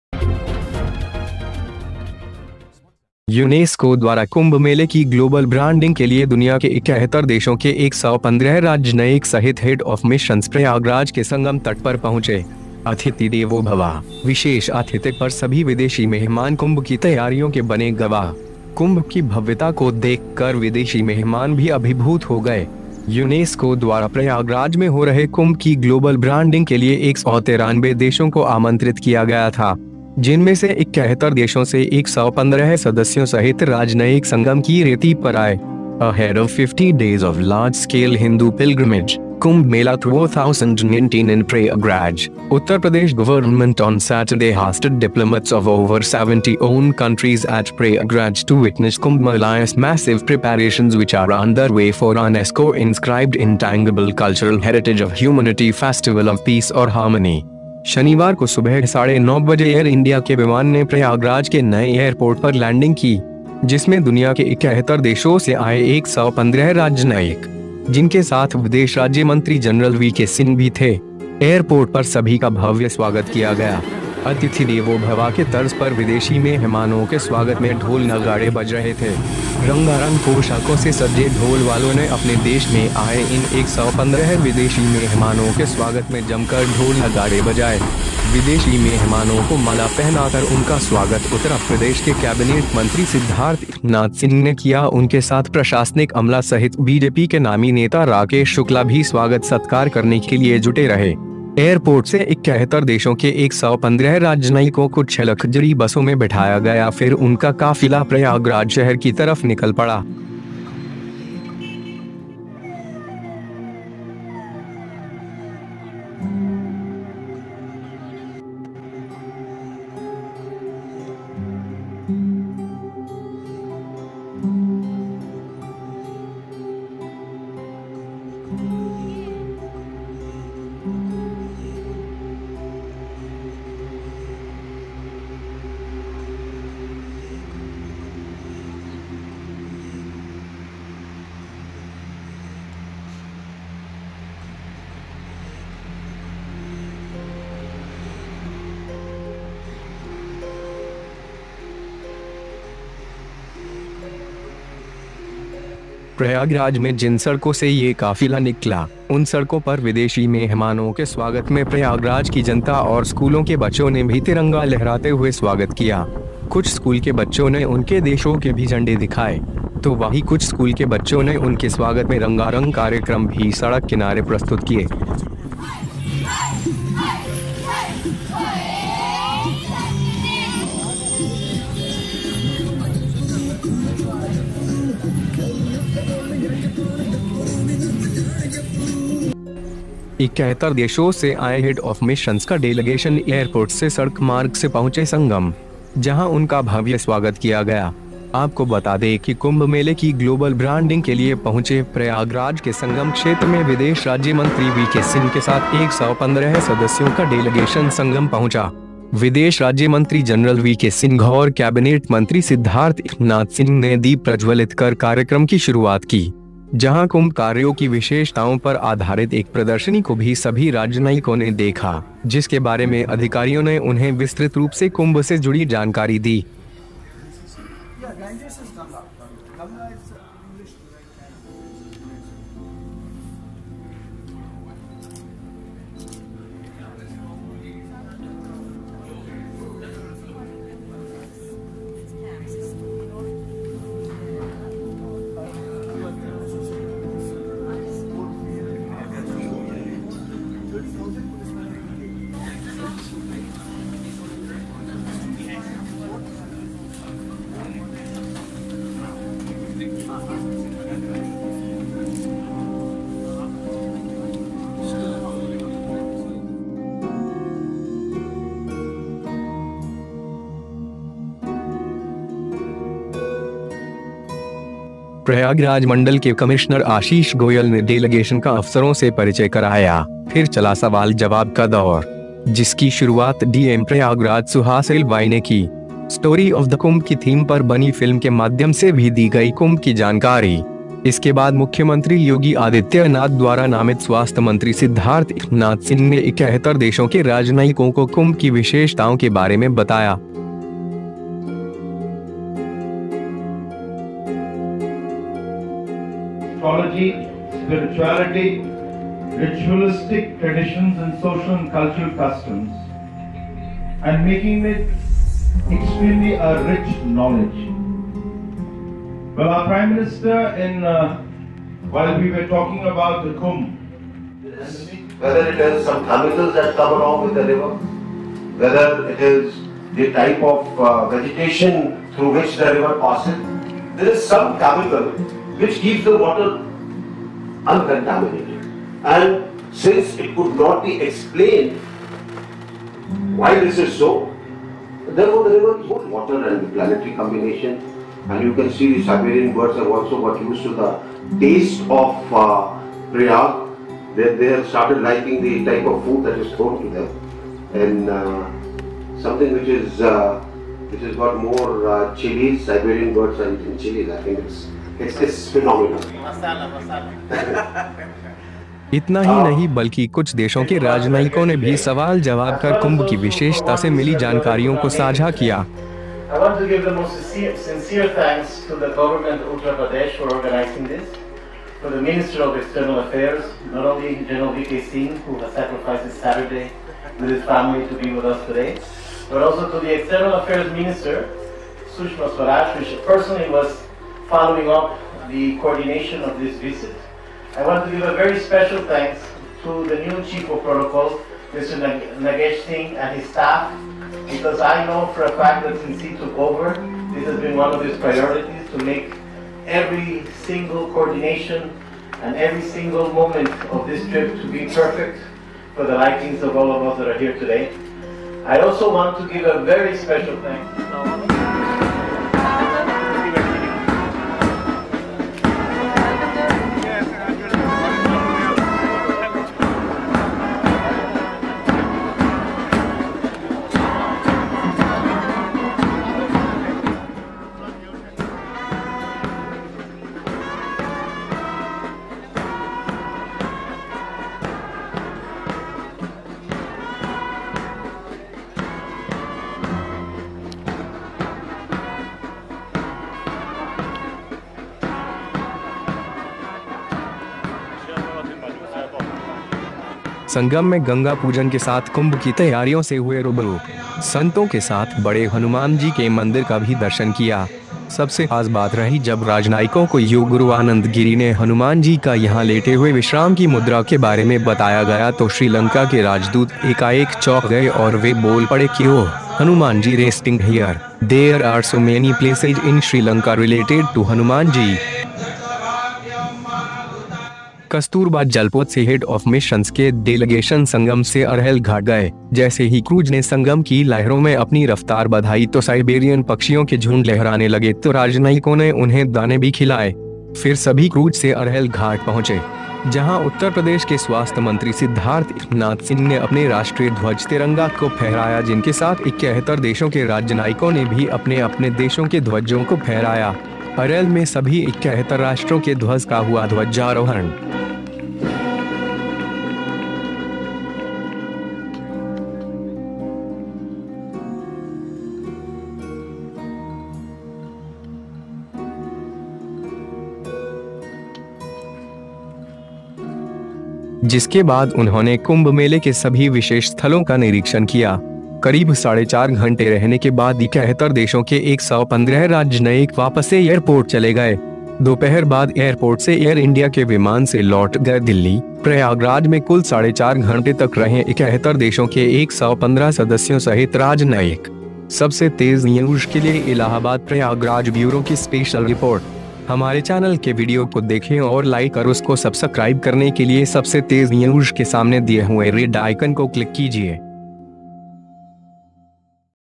यूनेस्को द्वारा दुनिया भर में हुई कुम्भ 2019 की ग्लोबल ब्रांडिंग
यूनेस्को द्वारा दुनिया भर में कुम्भ 2019 की ग्लोबल ब्रांडिंग 71 देशों के 115राजनयिकों ने देखी कुम्भ की विशेषताएं।
यूनेस्को द्वारा कुम्भ मेले की ग्लोबल ब्रांडिंग के लिए
 दुनिया के इकहत्तर देशों के एक सौ पन्द्रह राजनयिक सहित हेड ऑफ मिशन्स
प्रयागराज के संगम तट पर पहुंचे। अथिति देवो भवा, विशेष आथित्य पर सभी विदेशी मेहमान कुम्भ की तैयारियों के बने गवाह। कुम्भ की भव्यता को देख कर विदेशी मेहमान भी अभिभूत हो गए।
 यूनेस्को द्वारा प्रयागराज में हो रहे कुम्भ की ग्लोबल ब्रांडिंग के लिये एक सौ तिरानबे देशों को आमंत्रित किया गया था। जिनमे से इकहत्तर देशों से एक सौ पन्द्रह सदस्यों सहित राजनयिक संगम की रेती पर आए।

      Ahead of fifty-days of large-scale Hindu pilgrimage, Kumbh Mela two thousand ninteen in Prayagraj, Uttar Pradesh government on Saturday hosted diplomats of over seventy one countries at Prayagraj to witness Kumbh Mela’s massive preparations which are underway for UNESCO inscribed ‘Intangible Cultural Heritage of Humanity’ festival of peace & harmony.

 शनिवार को सुबह साढ़े नौ बजे एयर इंडिया के विमान ने प्रयागराज के नए एयरपोर्ट पर लैंडिंग की। जिसमे दुनिया के इकहत्तर देशो से आये एक सौ पन्द्रह राजनयिक। जिनके साथ विदेश राज्य मंत्री जनरल वी के सिंह भी थे। एयरपोर्ट पर सभी का भव्य स्वागत किया गया। अतिथि देवो भवा के तर्ज पर विदेशी मेहमानों के स्वागत में ढोल नगाड़े बज रहे थे। रंगारंग पोशाकों से सजे ढोल वालो ने अपने देश मे आये इन एक सौ पन्द्रह विदेशी मेहमानों के स्वागत में जमकर ढोल नगाड़े बजाए। विदेशी मेहमानों को माला पहनाकर  उनका स्वागत उत्तरप्रदेश के कैबिनेट मंत्री सिद्धार्थ नाथ सिंह ने किया उनके साथ प्रशासनिक अमला सहित बीजेपी के नामी नेता राकेश शुक्ला भी स्वागत सत्कार करने के लिए जुटे रहे।

एयरपोर्ट से इकहत्तर देशों के एक सौ पन्द्रह राजनयिकों को छह लक्जरी बसो में बिठाया गया फिर उनका काफिला प्रयागराज शहर की तरफ निकल पड़ा।

 प्रयागराज में जिन सड़को से ये काफिला निकला, उन सड़कों पर विदेशी मेहमानों के स्वागत में प्रयागराज की जनता और स्कूलों के बच्चो ने भी तिरंगा लहराते हुए स्वागत किया। कुछ स्कूल के बच्चो ने उनके देशो के भी झंडे दिखाए। तो वही कुछ स्कूल के बच्चो ने उनके स्वागत में रंगारंग कार्यक्रम भी सड़क किनारे प्रस्तुत किये।

इकहत्तर देशो से आये हेड आफ मिशन्स  का डेलिगेशन एयरपोर्ट से सड़क मारर